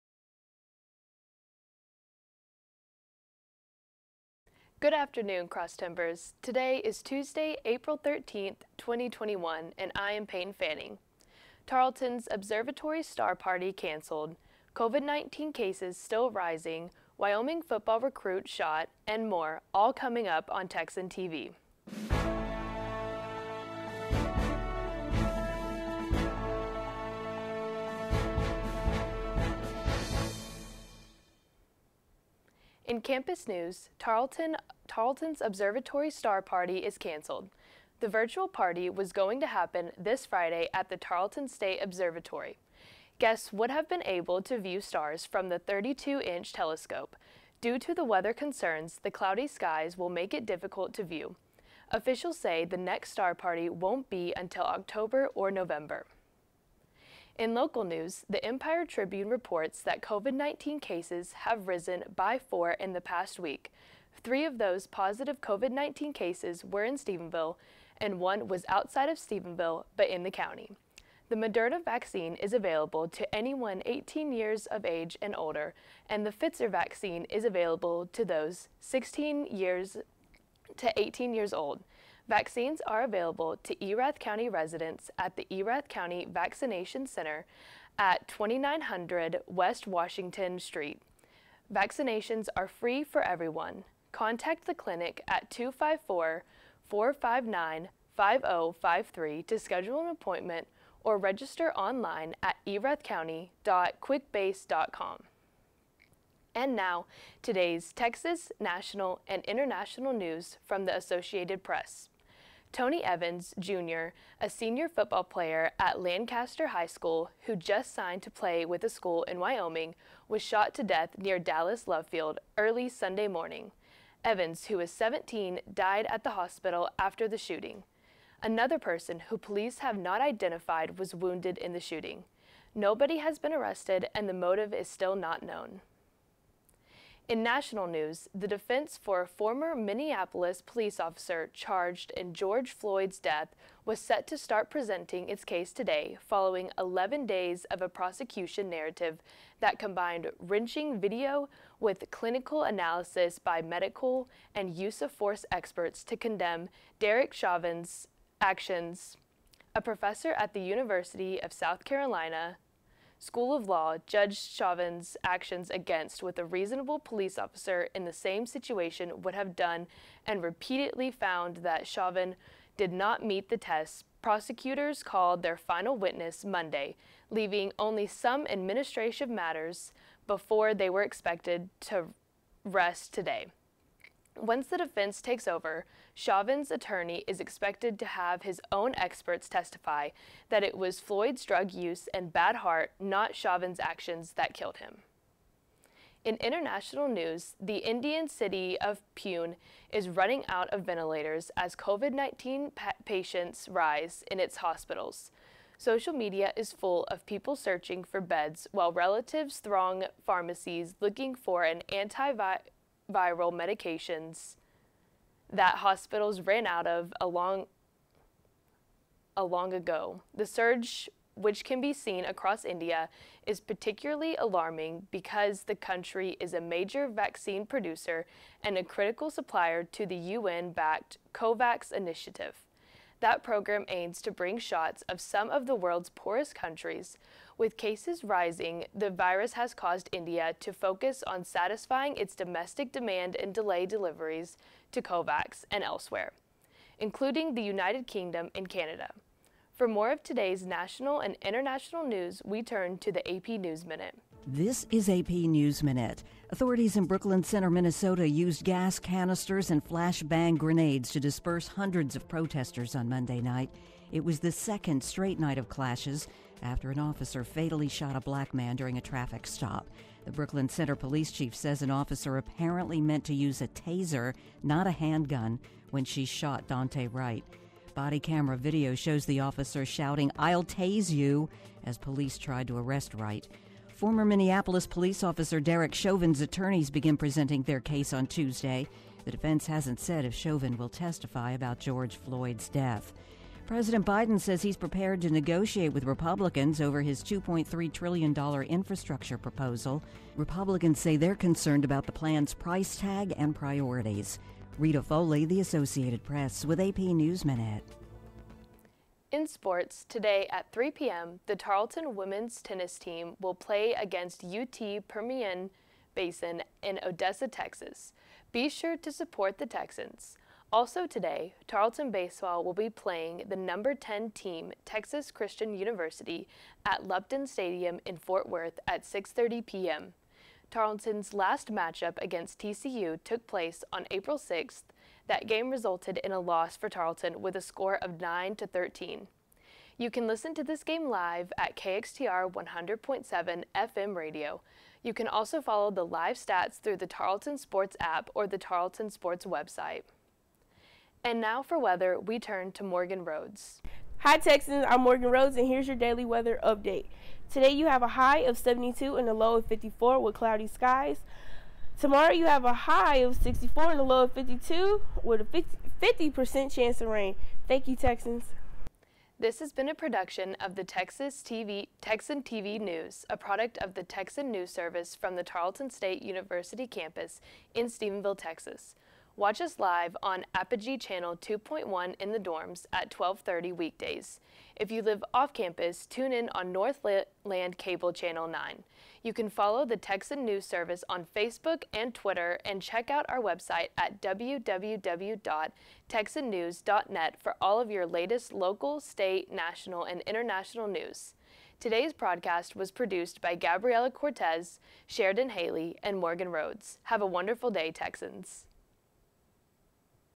Class Broadcast April 13, 2021 | Anchor: Peyton Fanning
Coming up on Texan TV: Tarleton’s observatory star party cancelled, Covid-19 cases still rising, Wyoming football recruit shot and more.